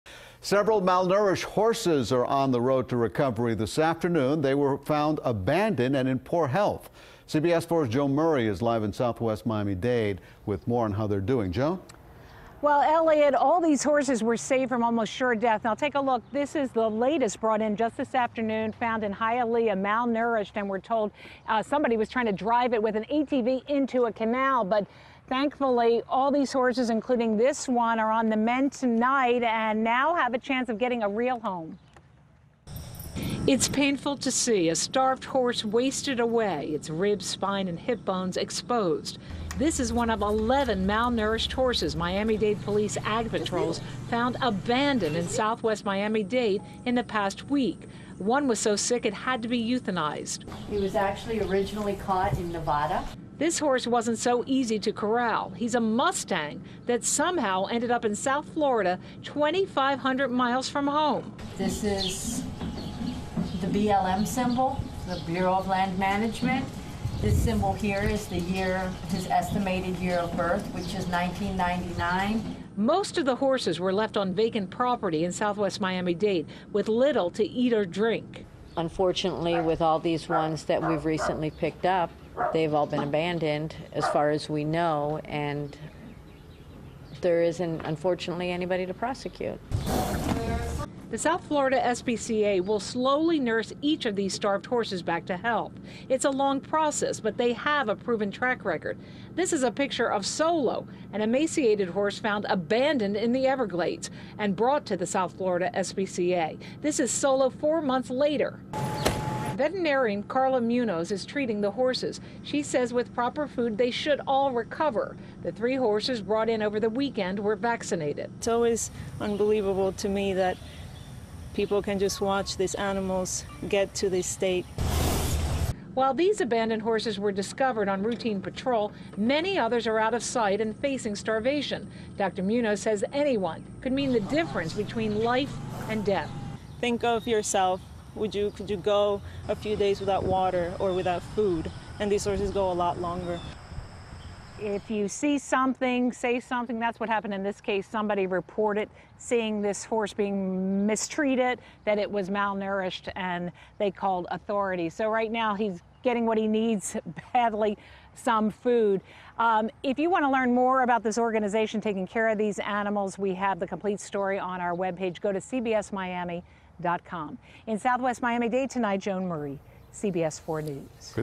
Nearly A Dozen Horses Found Malnourished In Dade
CLICK To Watch Joan Murray’s Report